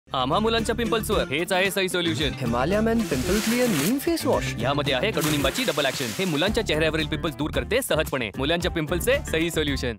The right solution to Men’s pimples (10s MARATHI)
To all the boys: Stop experimenting with weird pimple solutions & don’t think it will go away on its own. Try Himalaya Men Pimple Clear Neem Face Wash. It is loaded with the Double Action of Neem to solve men’s pimples!